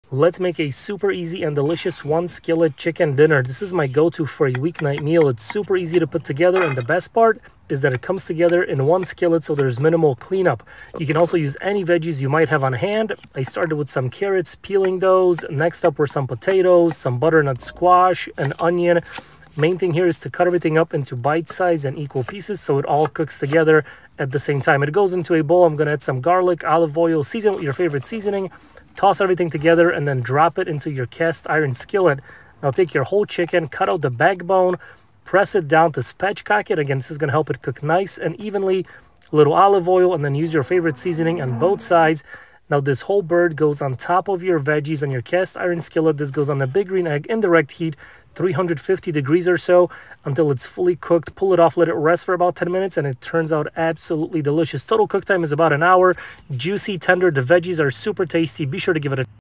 Easy One SKILLET Dinner
shortsmaschallenge Easy and delicious one skillet chicken dinner. These are perfect for quick weekday meals. And a great way to use up any vegetables you might have.